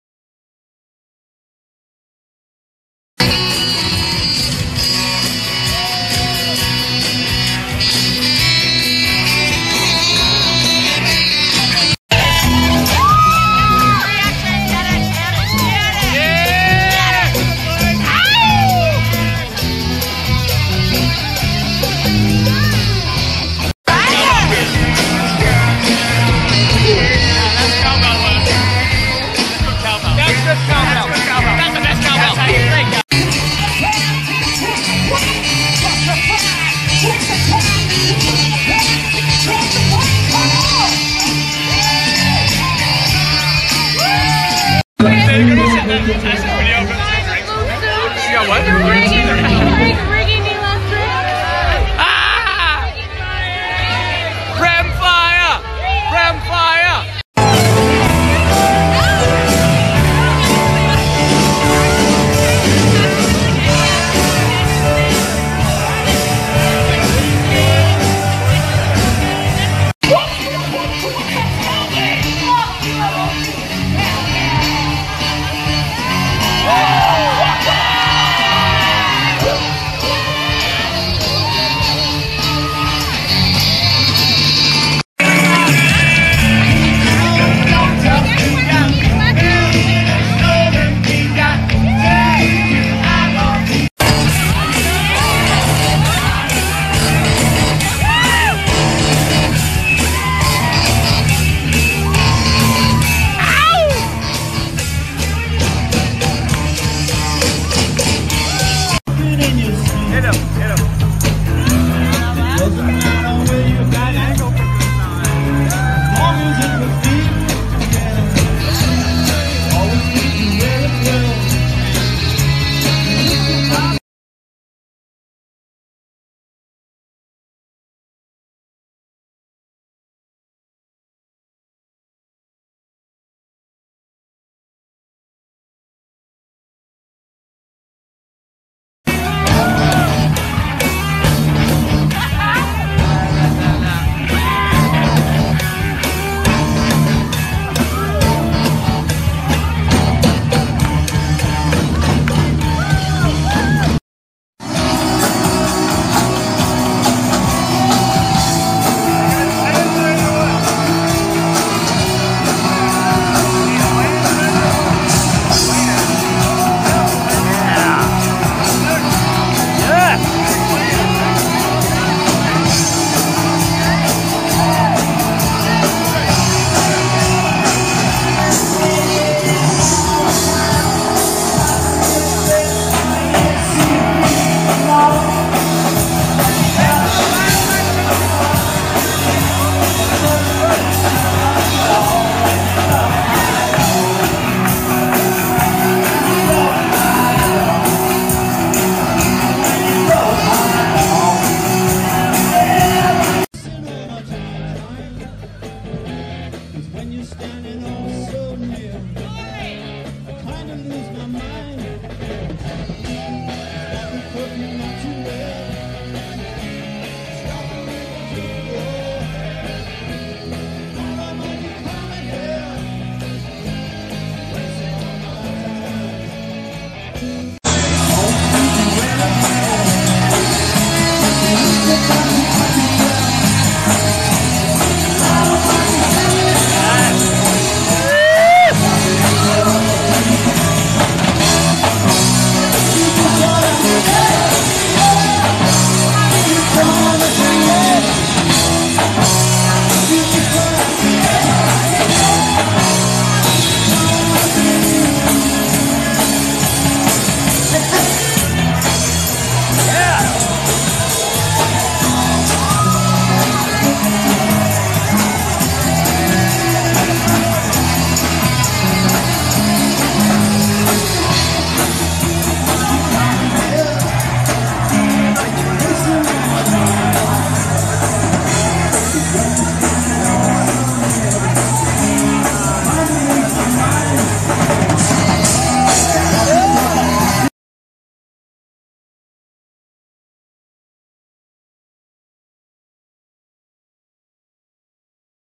Marie Avgeropoulos and Praimfaya In The 4th Annual Hunkerdown XMAS Showdown November 25th, 2017
The 100's Marie Avgeropoulos and her The 100 band Praimfaya rocked the house in Vancouver, which benefited Instruments Beyond Borders, Vancouver Film Industry, and the Safe Injection Site.

Credits: Sachin Sahel, Tasya Teles, and Marie Avgeropoulos

The 100 will air season 5 in 2018